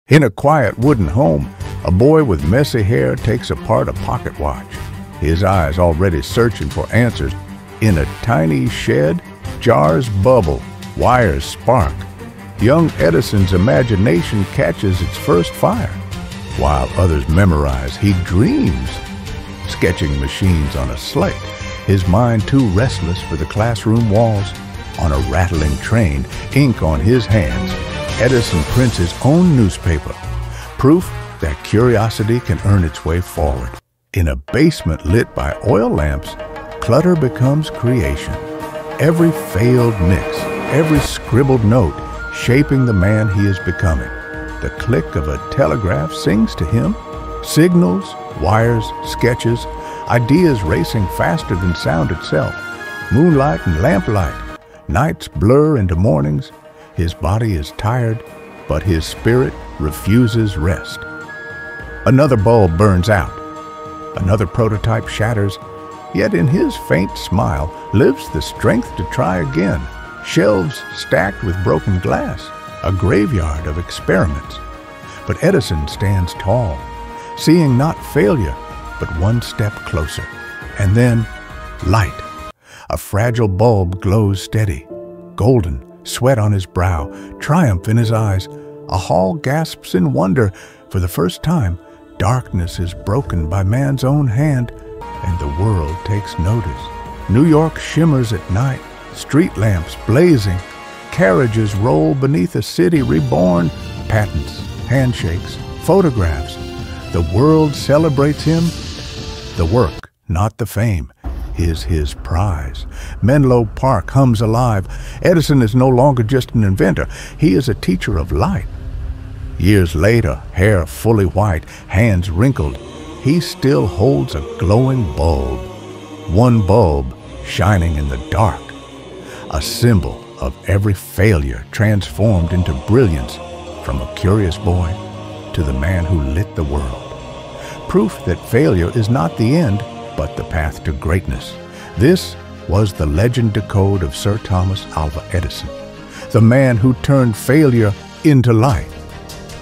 Sir Thomas Edison: From 1,000 Failures To Light
From a curious boy dismantling watches… to the man who lit up the world.
This is the journey of Thomas Alva Edison — the legend who turned failures into the power of light.
A story of grit, vision, and never giving up.

Moral : Failure is not the end — it’s the path to greatness.
Motive : To ignite belief in every dreamer: no matter how many times you fall, your light will shine if you refuse to quit.